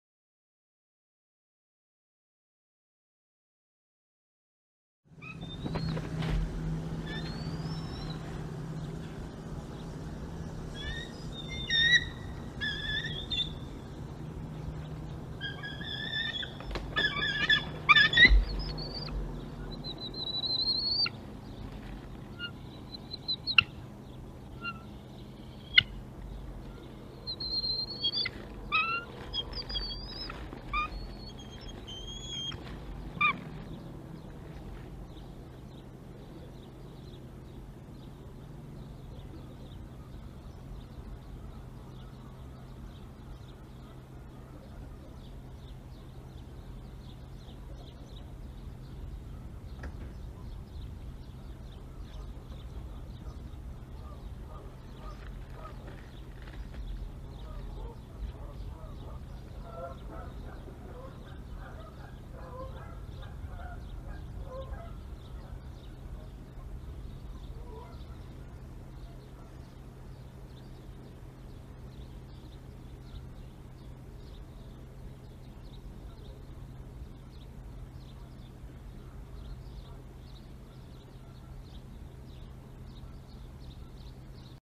February 17, 2021: Hasenpfeffer for lunch!
DM2 brings a frozen rabbit into nest N2B, with Mom hot on his tailfeathers! She lands, mantles, screes, and moves in, pushing DM2 off his prize. He gives it up and stands sentinel while Mom enjoys some hasenpfeffer for lunch.

We don't show it here, but DM2 caught the rabbit earlier and ate quite a bit of it himself before bringing the leftovers to N2B. Eagles establish a pecking order in the nest, learning when (and how) to dominate and when to stand down and let someone else eat. DM2 had already enjoyed a little lunch and was willing to surrender the rest of the meal to his hungry mate.